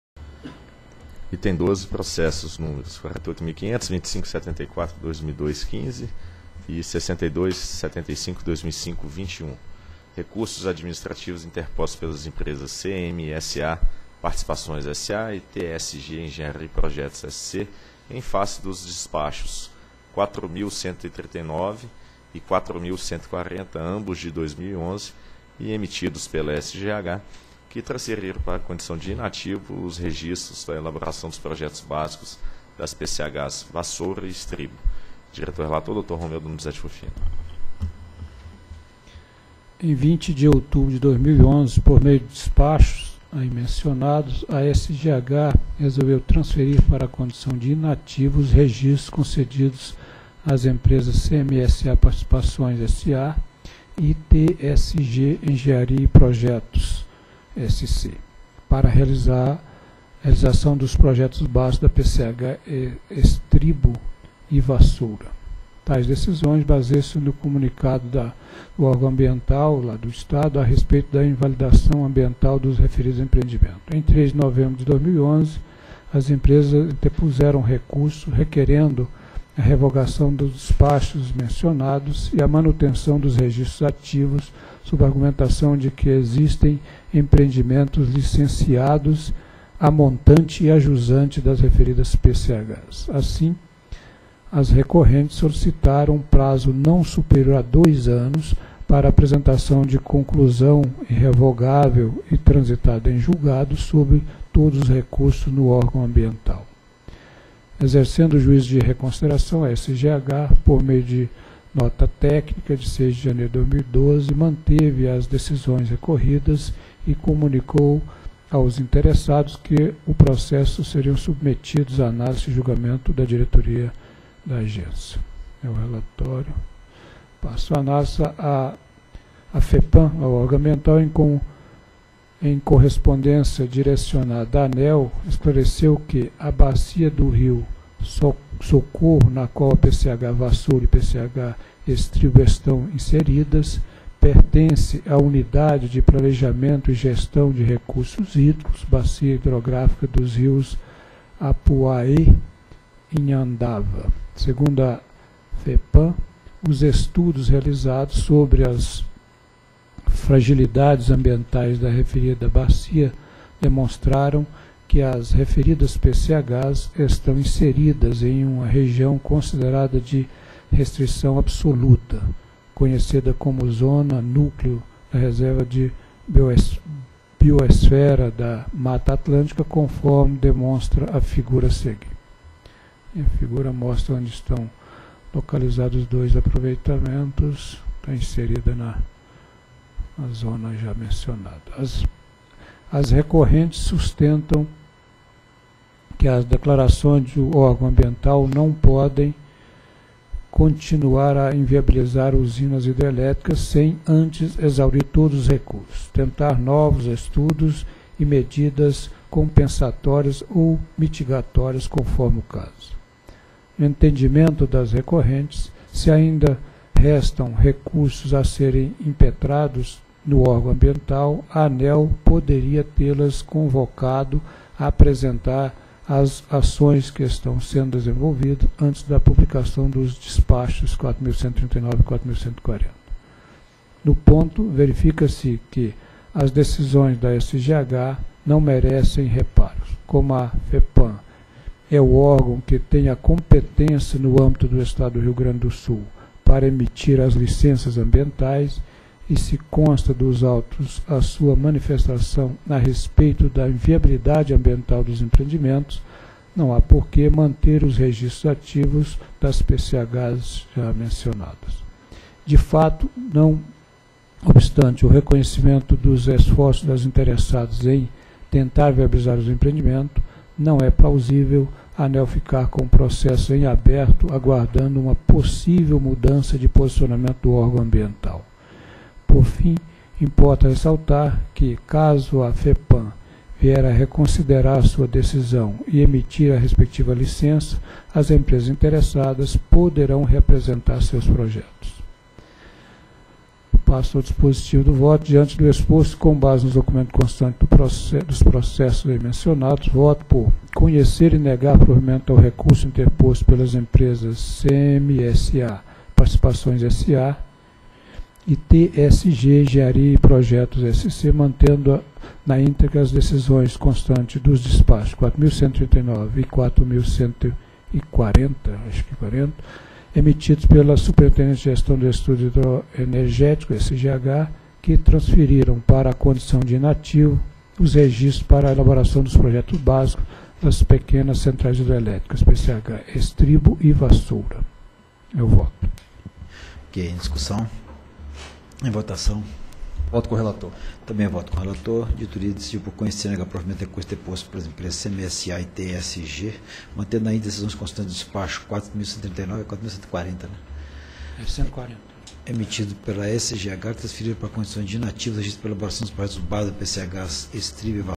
Item 12 - 5ª Reunião Pública Ordinária ANEEL 14/02/2012
Item 12. Processos nº: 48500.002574/2002-15 e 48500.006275/2005-21. Assunto: Recursos Administrativos interpostos pelas empresas CMSA Participações S.A. e TSG Engenharia e Projetos S/C em face dos Despachos nºs 4.139/2011 e 4.140/2011, emitidos pela Superintendência de Gestão e Estudos Hidroenergéticos -- SGH, que transferiram para a condição de inativo os registros para a elaboração dos projetos básicos das Pequenas Centrais Hidrelétricas -- PCHs Vassoura e Estribo. Área Responsável: Diretoria -- DIR.